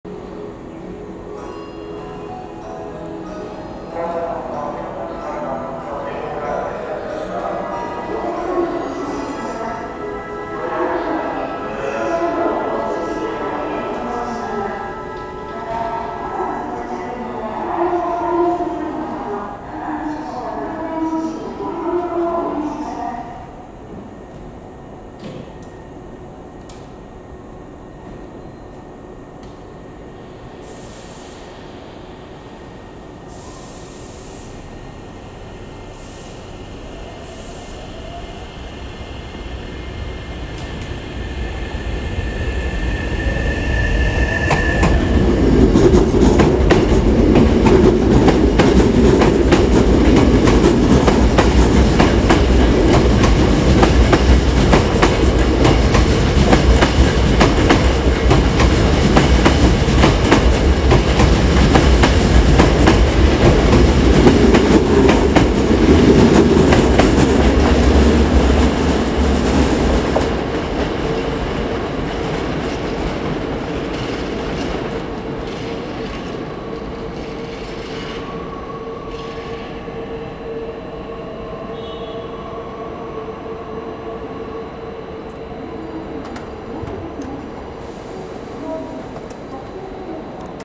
【同じ顔3本並びの同時発車】阪急9000系9002F,9005F,9300系9303F
2020年12月7日撮影。
12時ちょうど発綺麗な3本同じ顔並びの同時発車でした。